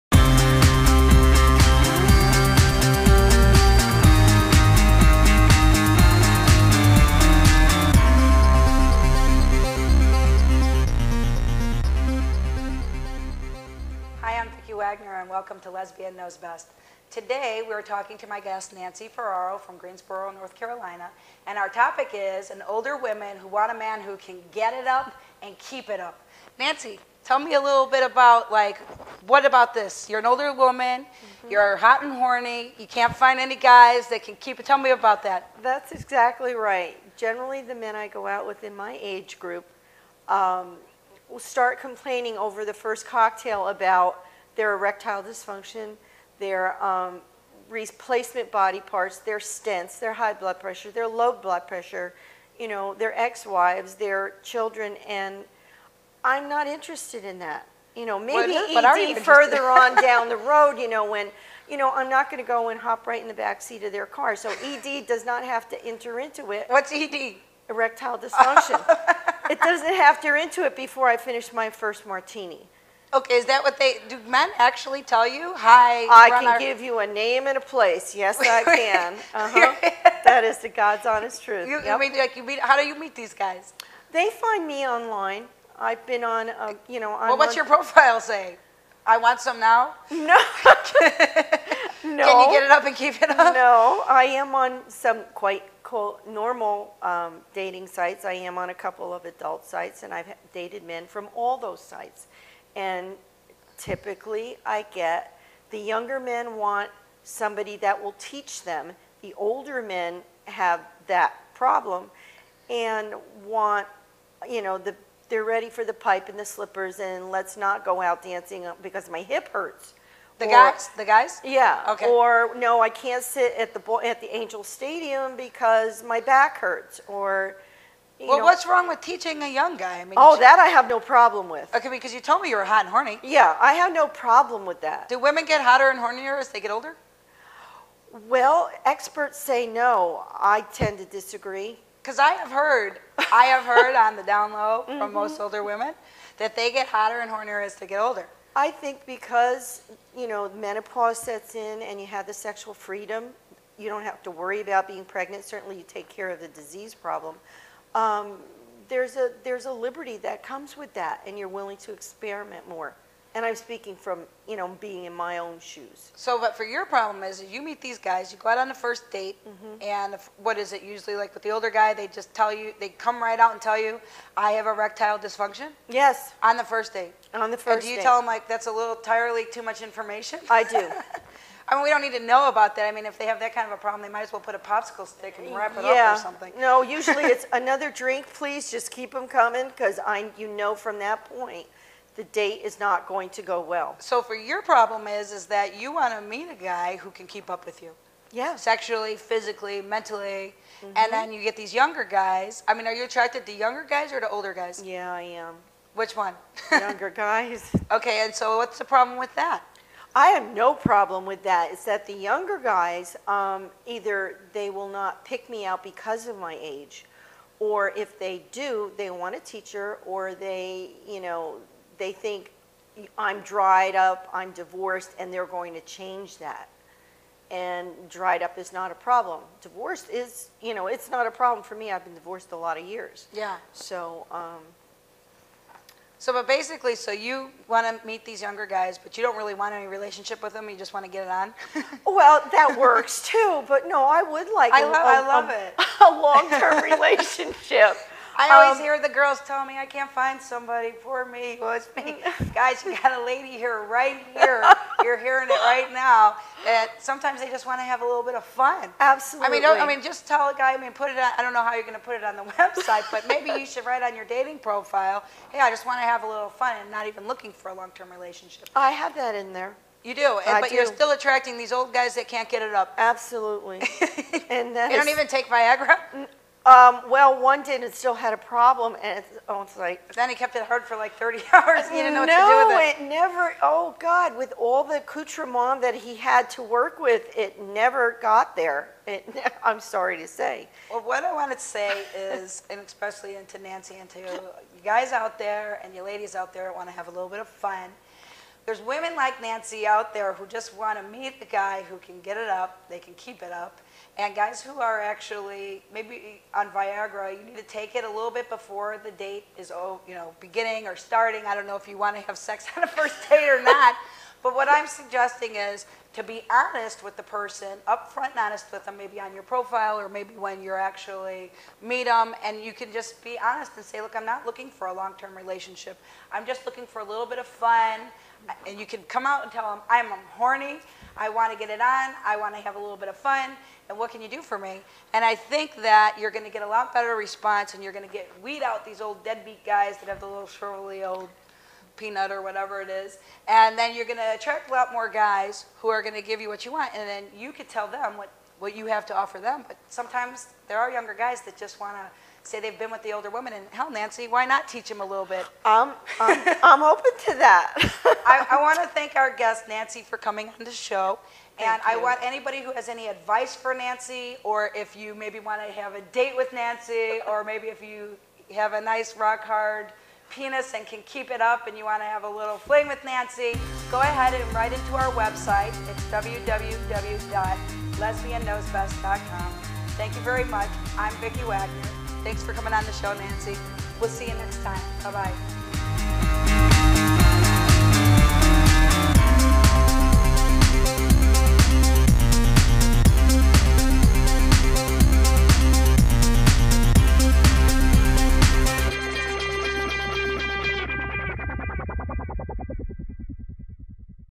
VIAGRA DOESN'T WORK!
Vicki Wagner talking to a woman who says that all older really want is a man who can get it up and keep it up and that sometimes they aren't looking for relationships at all.
Guest Nancy Ferraro